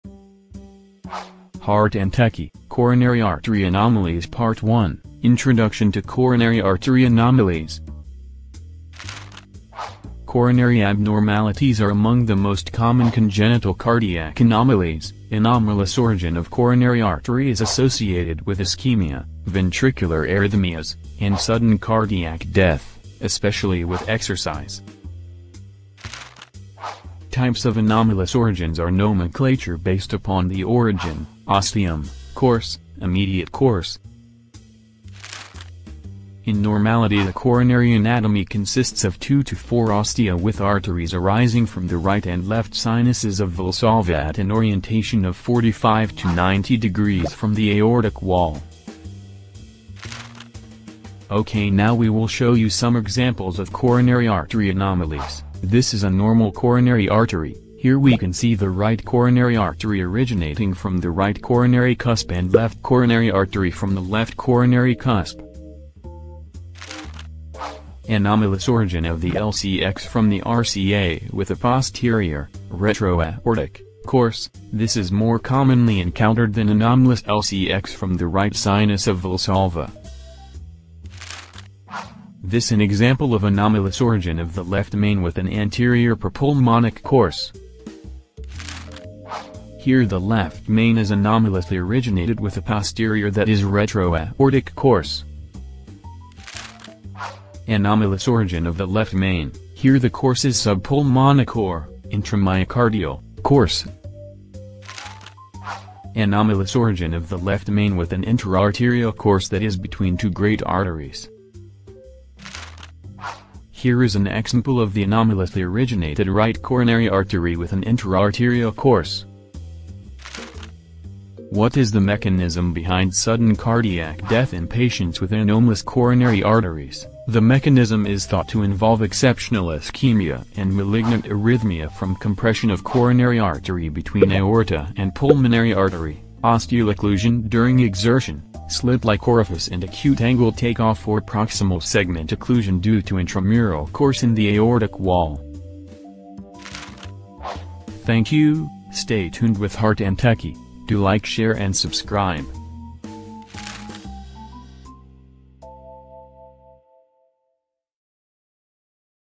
coronary artery anomalies
WELCOME TO OUR HEART AND TECHY FAMILY. : )

CORONARY ARTERY ANOMALIES PART 1

During this lecture, you will be able to learn how to assess Anomalies in the coronary artery in echocardiography. This is an introduction video, we will show you some examples of coronary artery anomalies in this video.We will try our best to convey our knowledge.

Enjoy the video...If you have any doubts about the topic or If we have anything to improve in our lecture please do comment.

STAY HOME .STAY SAFE. SAVE LIVES.

cardiac